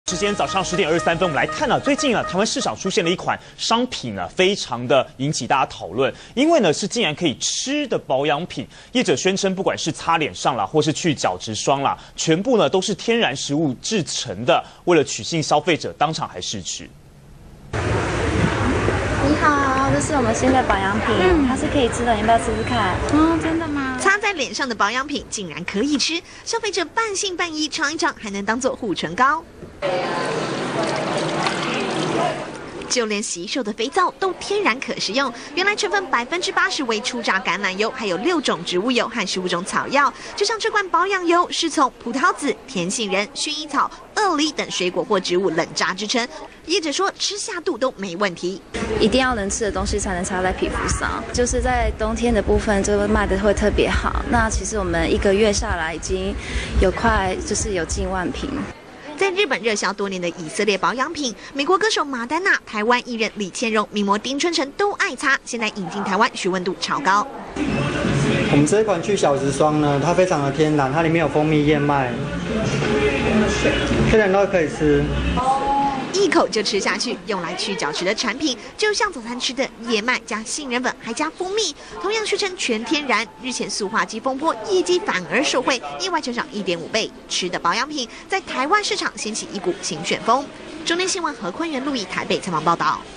保養品可擦也可吃！成分全是水果或植物
中天新聞》每年百貨週年慶都吸引許多民眾搶購保養品，不過今年台灣出現一款很特別的商品，就是「可以吃」的保養品，業者宣稱不管是面霜或去角質霜，都是全天然食物製成，為了取信消費者竟然還當場試吃。